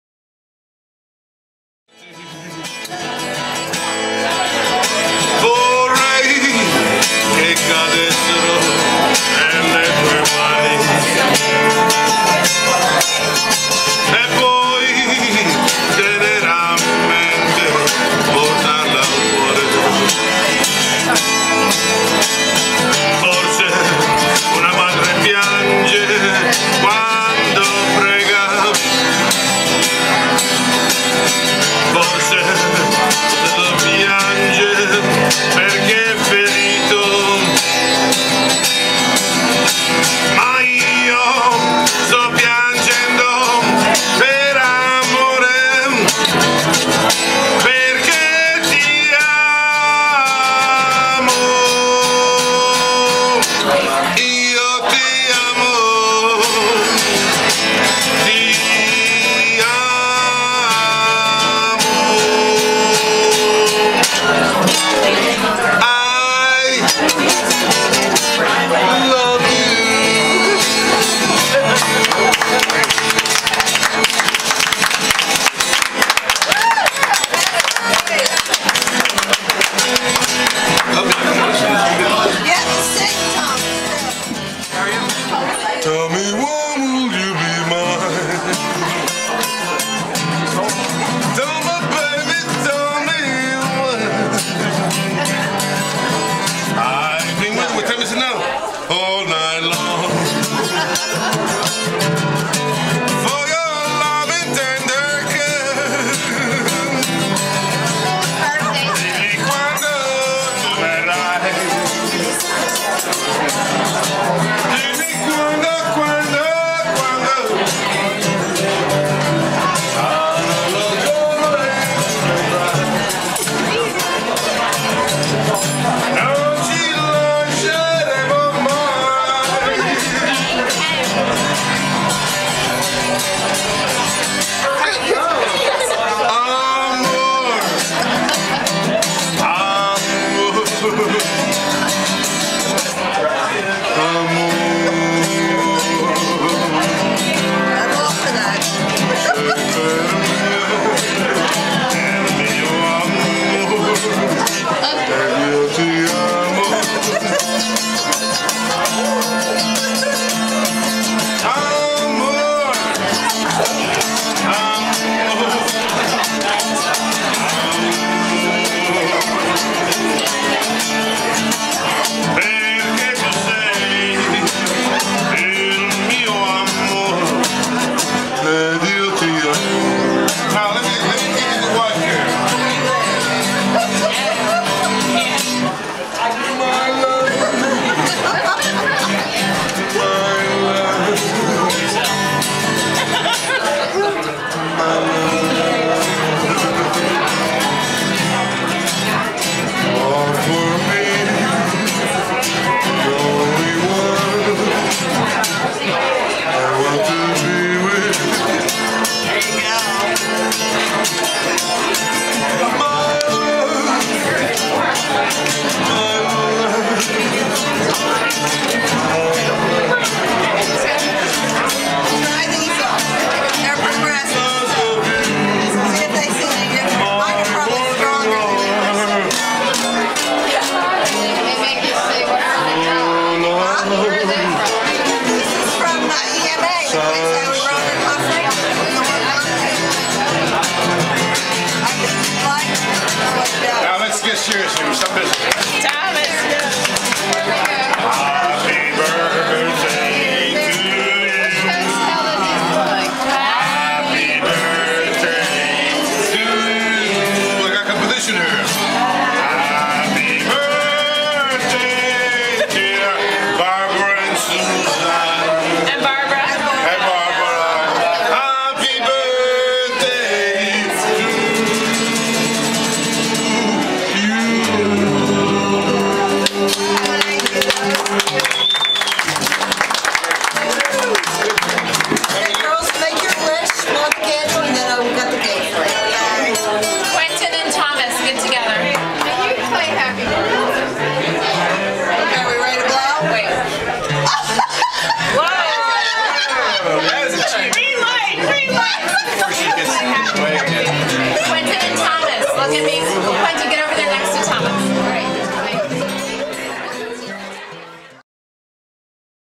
A family birthday celebration at D'Angelo's Ristorante Italiano
The Bowers/Ward family gathered at this renowned and charming eatery in Center City Philadelphia on Saturday, January 28, 2012 to celebrate the birthdays of Barbara Bowers, Barbara Ward, and Susan Bowers.  It was lovely evening, made more special by the serenading of Sal D'Angelo, one of the owners.